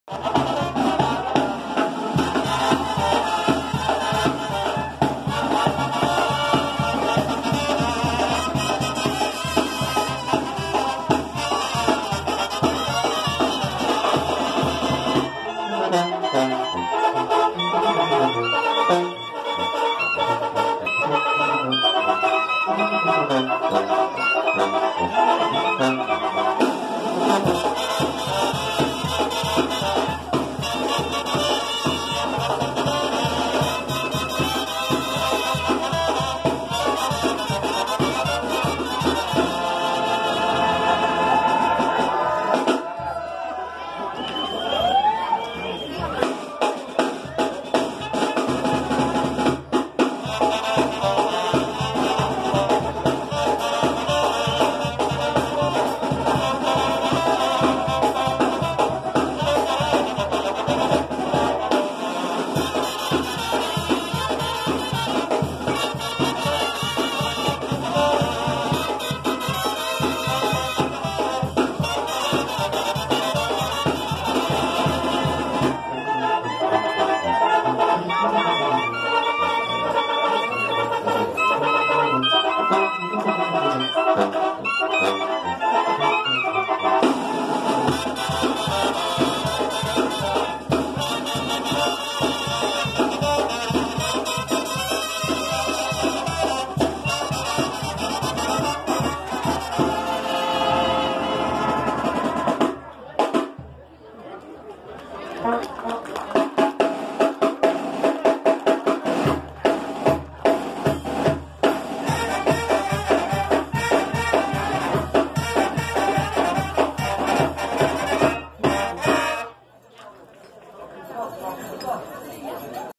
El Toro Pinto Dusveles vs Ermitaño 12 de Enero 2020
Fiesta Patronal en la ISLA URANDEN DE MORELOS 12 de Enero 2020